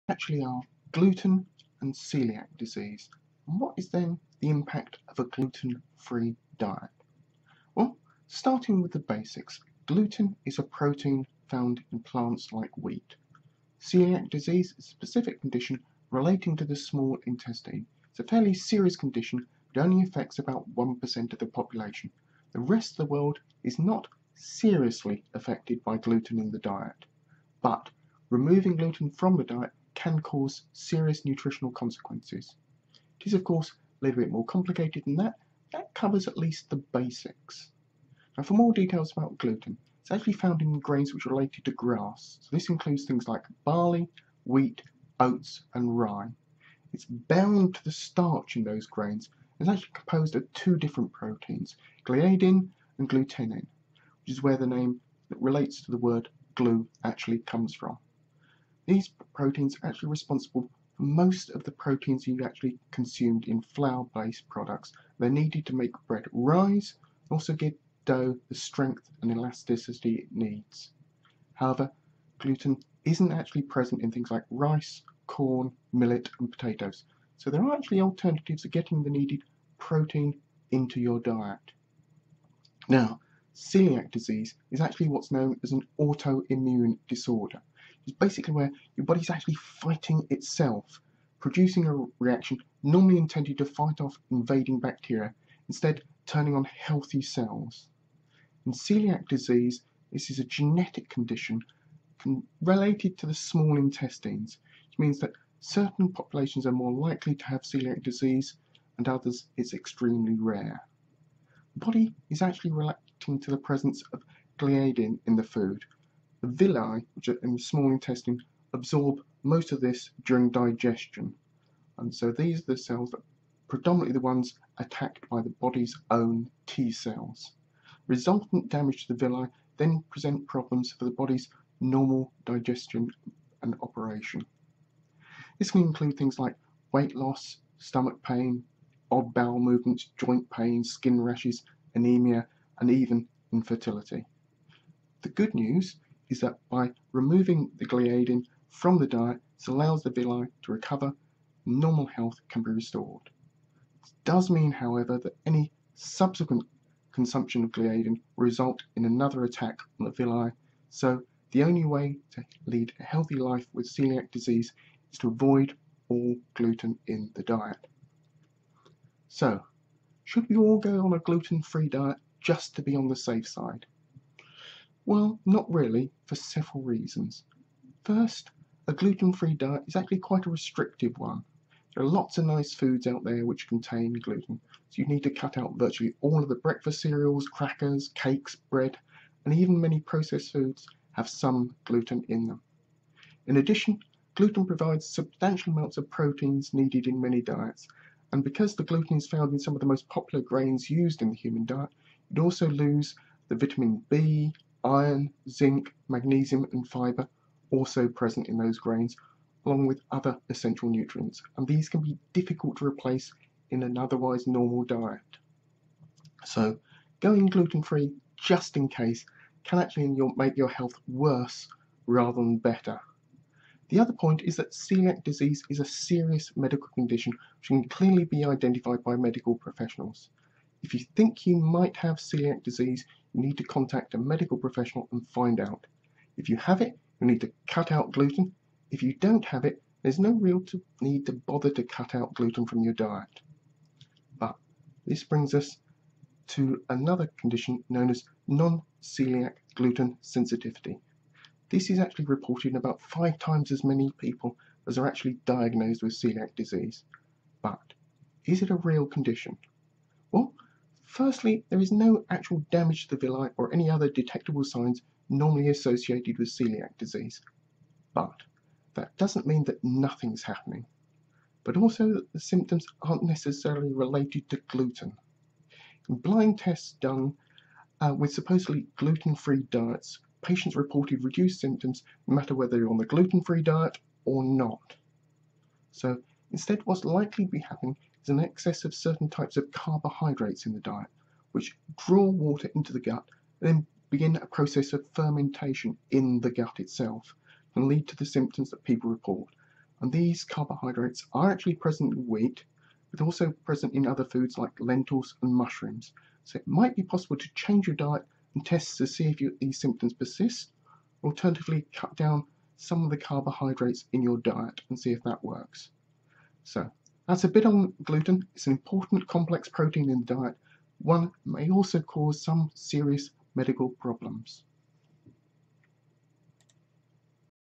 What are gluten and celiac (coeliac) disease?
A look at gluten, celiac disease and the impact of a gluten free diet? How gliadin one of the proteins in gluten is responsible in those who have the genetic condition of celiac disease for making t-cells attack the villi in the small intestine causing serious medical problems.